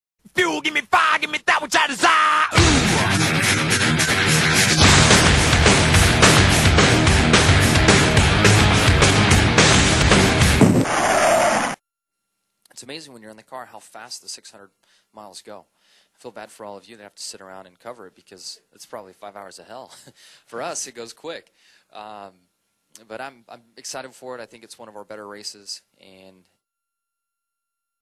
NASCAR At Lowes: Jimmie Johnson
Johnson on how 600 miles goes quickly for the drivers